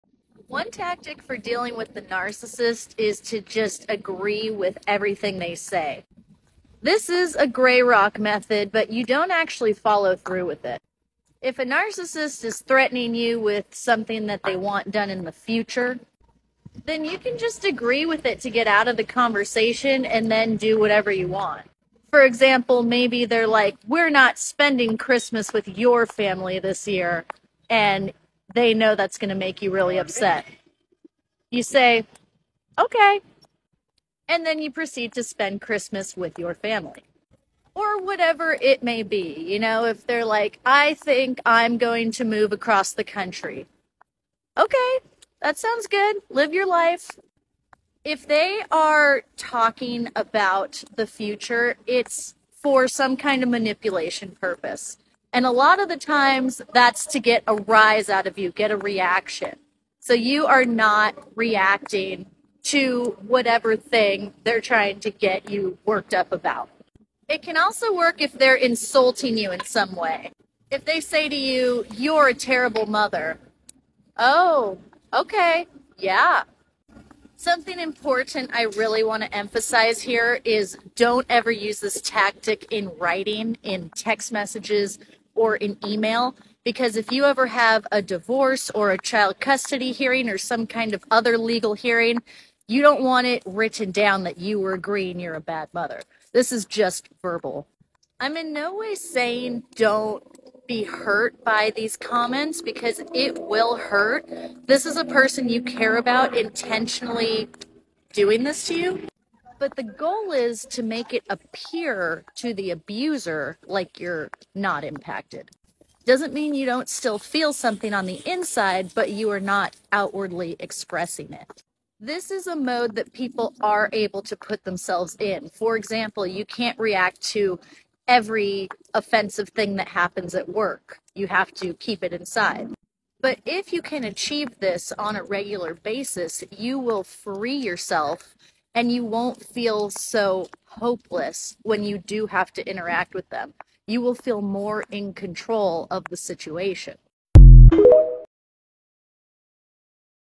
The “agree rock” tactic for dealing with a narcissist
“Agree rock” can be very freeing. (This tactic should only be used verbally and never written down.)